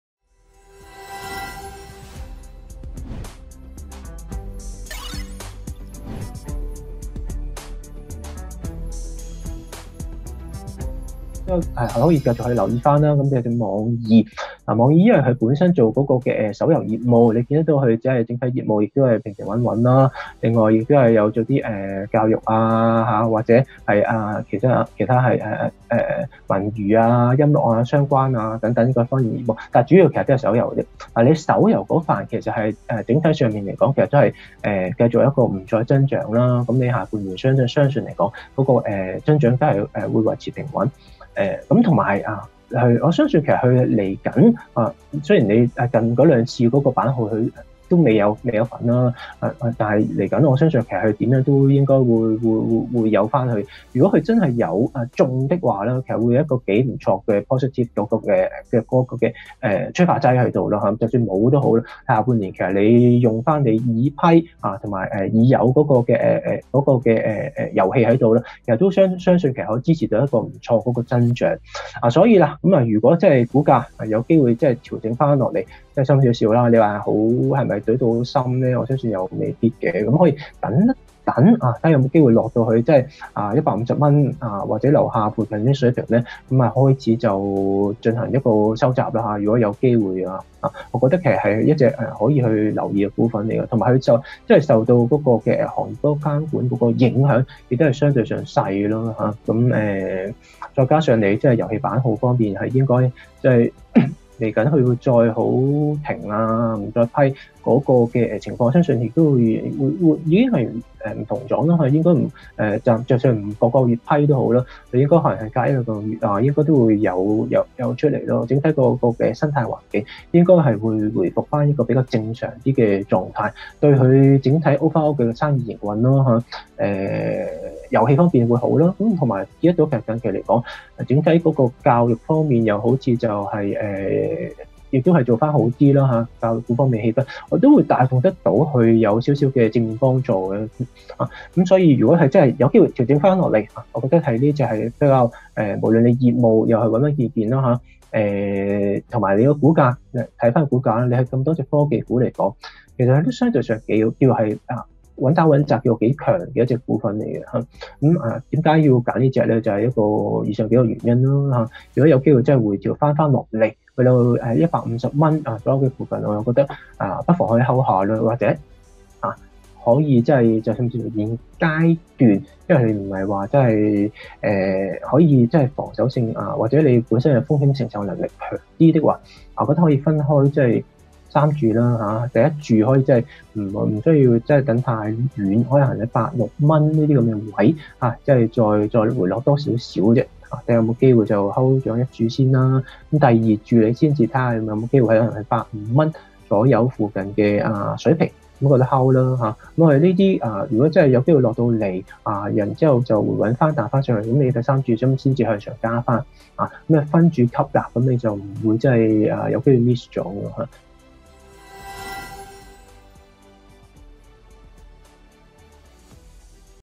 【潛力股精選】梁耀康：網易（09999）
網易（09999）收市報162.6元，跌5.74%。獨立股評人梁耀康指，網易整體業務平穩，雖然最新一批遊戲版號未獲批，但估計日後將會復批，而現有已批出的遊戲亦可支持業務增長，近期教育股造好亦可帶動網易向好。梁耀康建議投資者分3注吸納。立即點擊收看／收聽。→【即睇買賣策略】